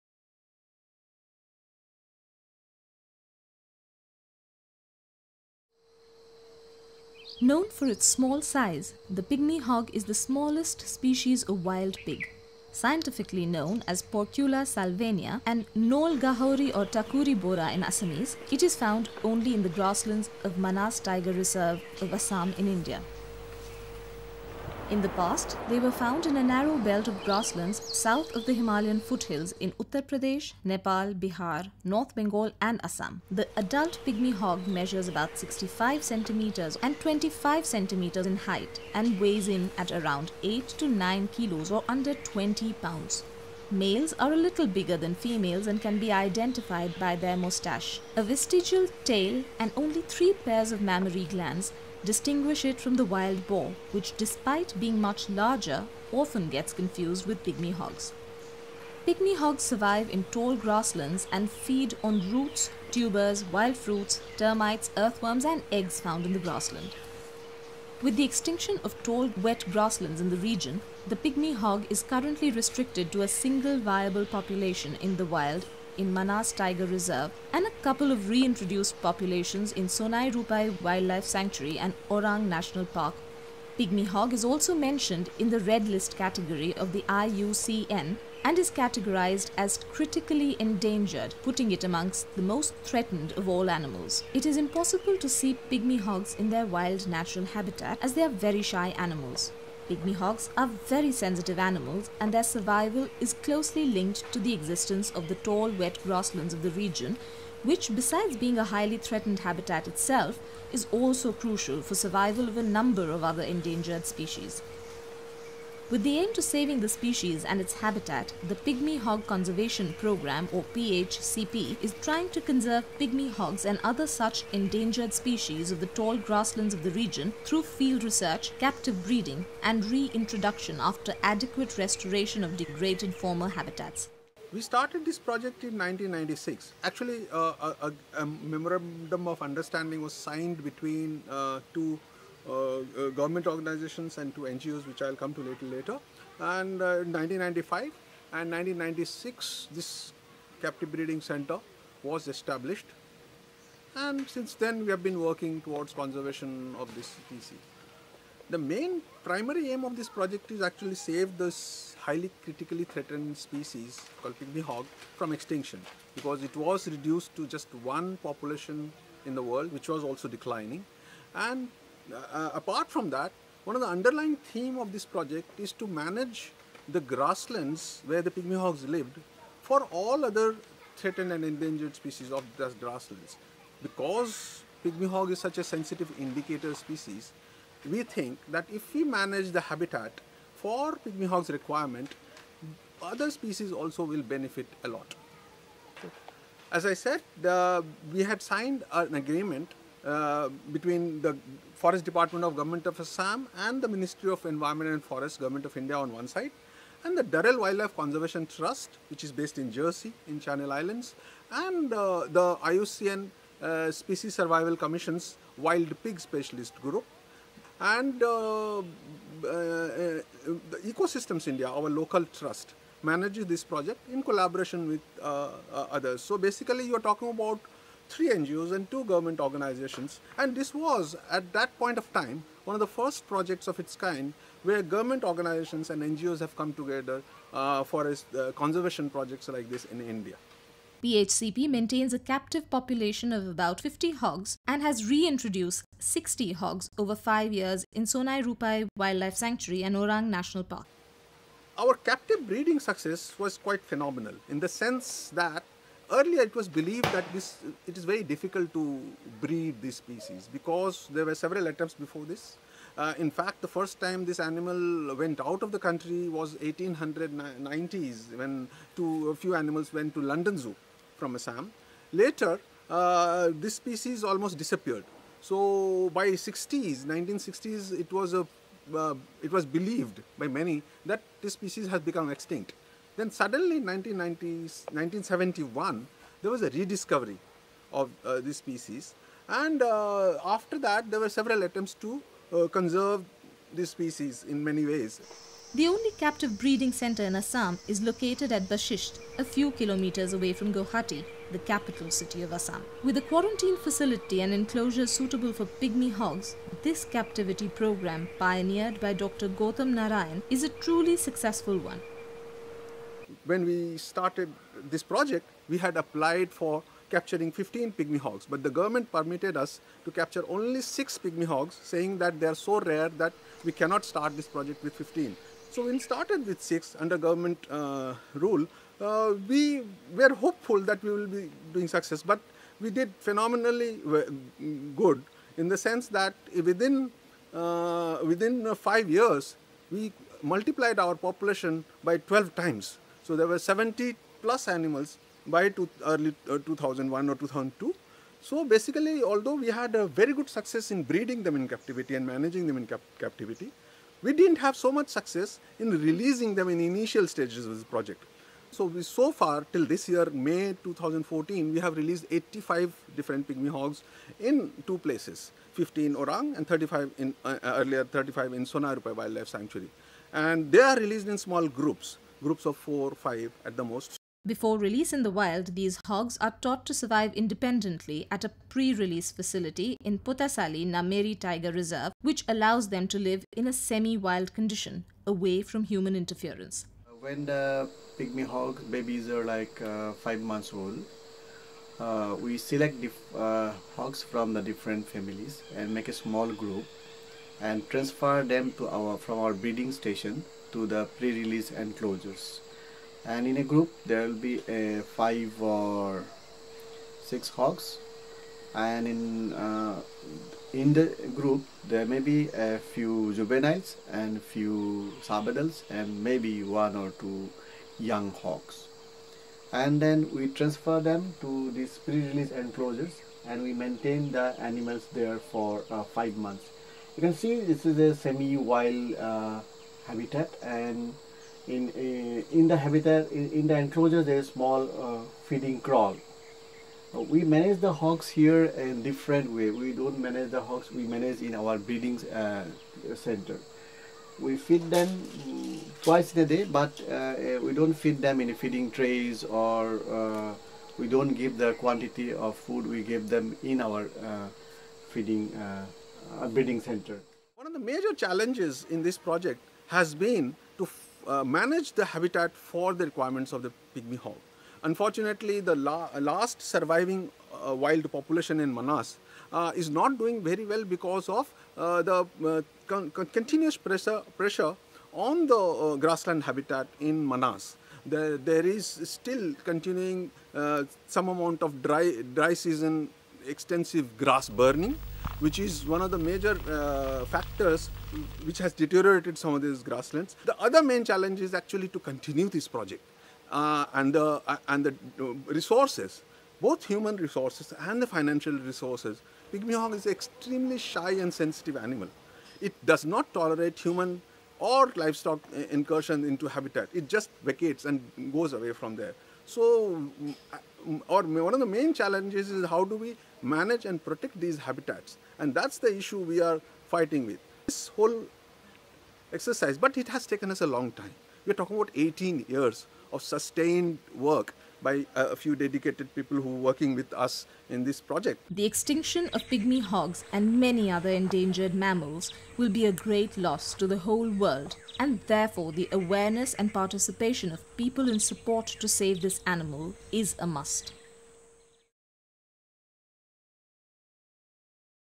Pygmy Hog saviours of India - only 150 of these tiny pigs remain in wild
Known for its small size, the Pygmy Hog is the smallest species of wild pig. Scientifically known as Porcula salvania and Nol Gahori or Takuri Borah in Assamese, it is found only in the grassland of Manas Tiger Reserve of Assam in India. In the past they were found in a narrow belt of grassland south of Himalayan foothills in Uttar Pradesh, Nepal, Bihar, North Bengal and Assam.

With features like that of a Wild boar, but relatively much smaller than even a baby wild boar, the adult Pygmy Hog measures about 65 cm (25 inches) in length and 25 cm (10 inches) in height and weighs in at around 8 to 9 kgs or under 20 pounds. Males are little bigger than females and can be identified by their moustache. Newborn babies weigh only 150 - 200 grams. A vestigial tail and only three pairs of mammary glands distinguish it from the wild boar, which, despite being much larger, often gets confused with pygmy hogs. Pygmy Hogs survive in tall grasslands and feed on roots, tubers, wild-fruits, termites, earthworms and eggs found in the grasslands.

With the extinction of tall, wet grasslands in the region, the Pygmy Hog is currently restricted to a single viable population in the wild in Manas Tiger Reserve and couple of reintroduced populations in Sonai Rupai Wildlife Sanctuary and Orang National Park, all in North-Western Assam. Pygmy Hog is also mentioned in the red list category of the IUCN and is categorized as ‘Critically Endangered,’ putting it among the most threatened of all mammals. Although accurate numbers are not known, it is estimated that there may be only a few hundred, probably less than 300, Pygmy Hogs left in the wild.

It is impossible to see Pygmy Hogs in their wild natural habitat, as they are very shy animals. They are well adapted to move fast through tall dense grasses with their streamlined bodies.
Pygmy Hogs are very sensitive animals and their survival is closely linked to the existence of the tall, wet grasslands of the region, which, besides being a highly threatened habitat itself, is also crucial for survival of a number of other endangered species.
With the aim to saving the species and its habitat the Pygmy Hog Conservation Programme (PHCP), a broad-based research and conservation programme is a collaborative project of the Durrell Wildlife Conservation Trust, IUCN/SSC Wild Pig Specialist Group, the Forest Department – Govt. of Assam and the Ministry of Environment & Forests – Govt. of India that has been trying to conserve Pygmy Hogs and other such endangered species of the tall grasslands of the region through field research, captive breeding and re-introduction after adequate restoration of degraded former habitats. It is administered in Assam by EcoSystems-India.

One of the main activities of PHCP is conservation breeding of pygmy hogs. PHCP maintains a captive population of about 50 hogs and has reintroduced 60 hogs over five years in Sonai Rupai Wildlife Sanctuary and Orang National Park. The only captive breeding center in Assam is located at Basistha, a few km’s away from Guwahati, the capital city of Assam. With a quarantine facility and enclosures suitable for Pygmy Hogs, this captivity programme pioneered by Dr. Goutam Narayan is a truly successful one. Before release in the wild, these hogs are taught to survive independently at a pre-release facility in Potasali, Nameri Tiger Reserve, which allows them to live in a semi-wild condition away from the human interference.

Extinction of Pygmy Hogs and many other endangered mammals will be a great loss to the whole world and therefore the awareness and participation of people in support to save this animal is a must.